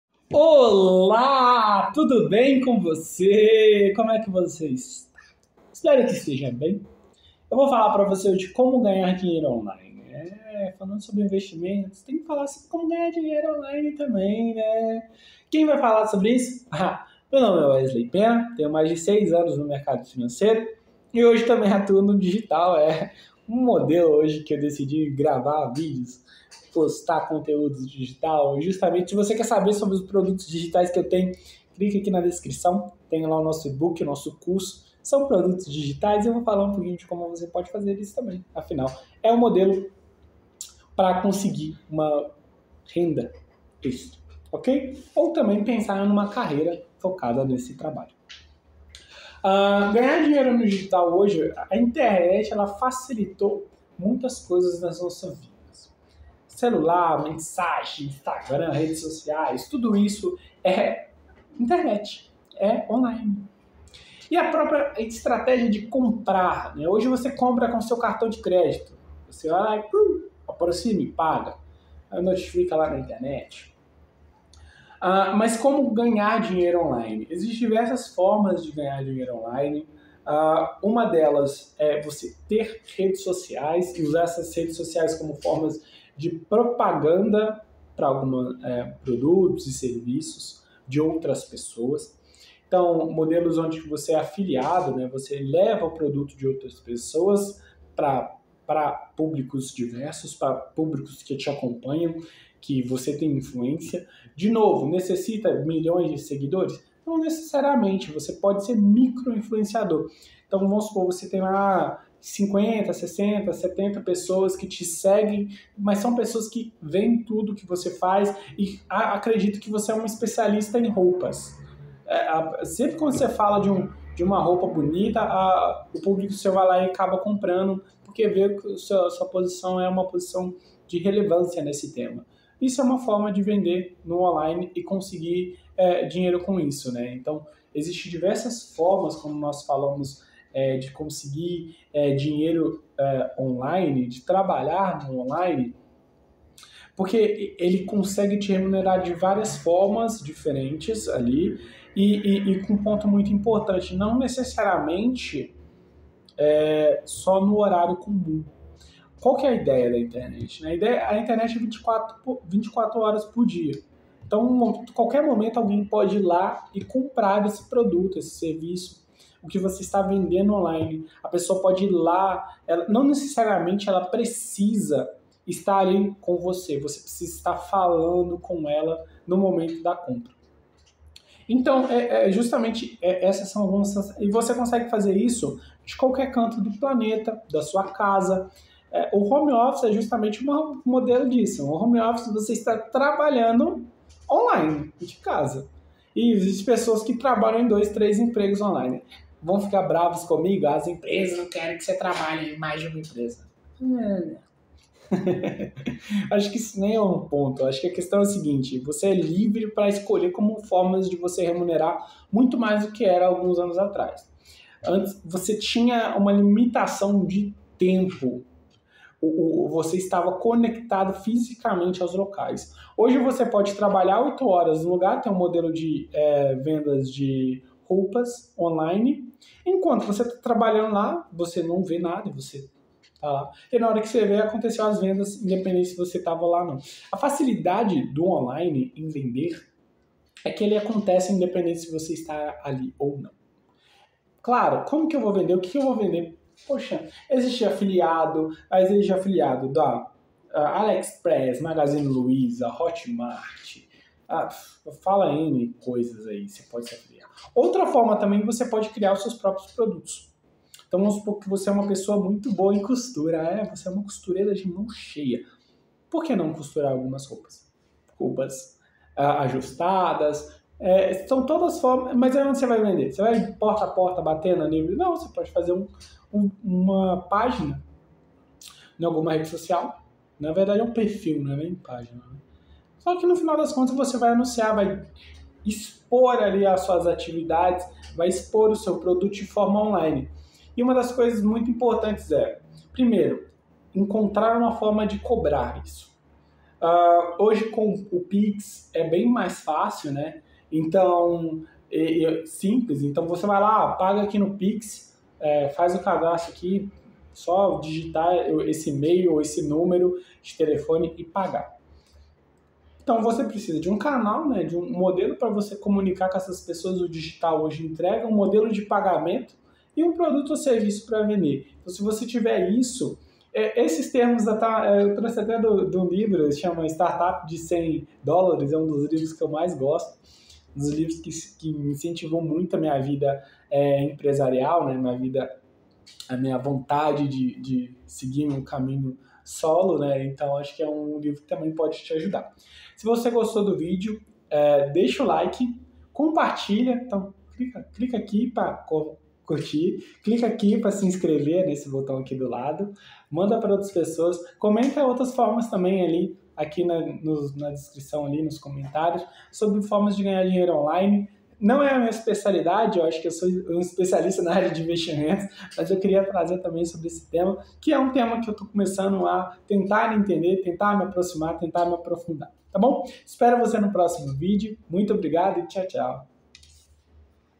Como ganhar dinheiro online: Ideias e métodos comprovados para aumentar sua renda na internet
Ideias e métodos comprovados para aumentar sua renda na internet

Você quer aprender mais sobre investimentos?

👉 Curso "Aprendendo sobre Investimentos": Um curso com certificado, que vai te ensinar tudo sobre o mundo dos investimentos.

👉 Clube "Aprendendo sobre Investimentos": Junte-se a uma comunidade de investidores e aprenda com os melhores.